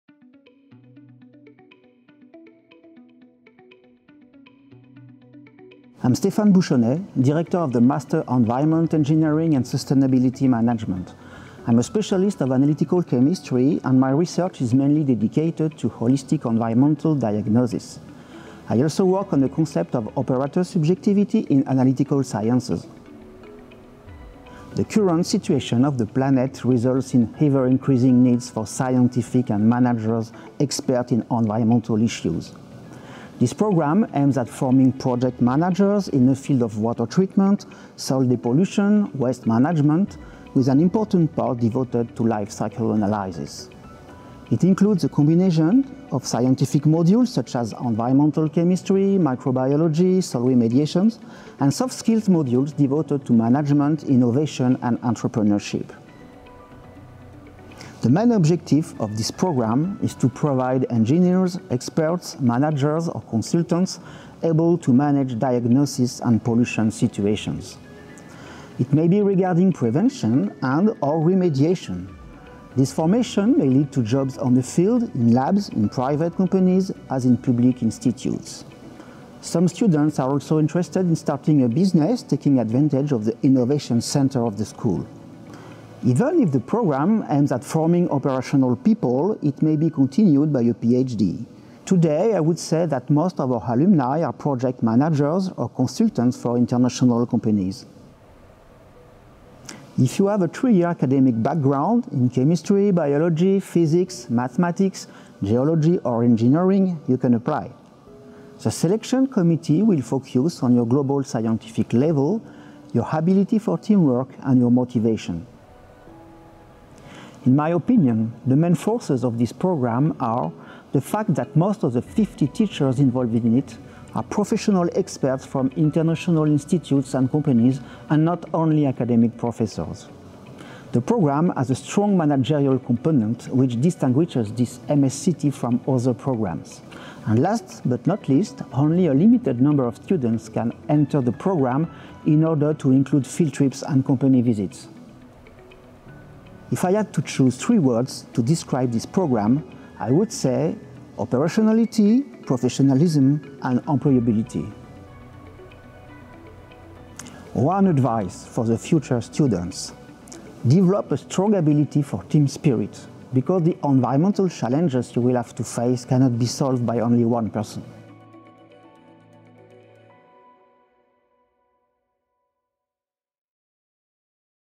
MSc&T Environmental Engineering and Sustainability Management
The Program provides students with real-world technical expertise on environmental issues, including soil and water pollution diagnosis, and treatment and valorization processes, as well as in-depth understanding of the economic and social challenges surrounding their development. In addition to the technical courses (very engineering oriented), students also study courses about international regulations, ethical issues, management, innovation and entrepreneurship.

The Master in Environmental Engineering and Sustainability Management (EESM) is involved in a number of valuable industrial partnerships, and offers opportunities for internships and international academic exchanges.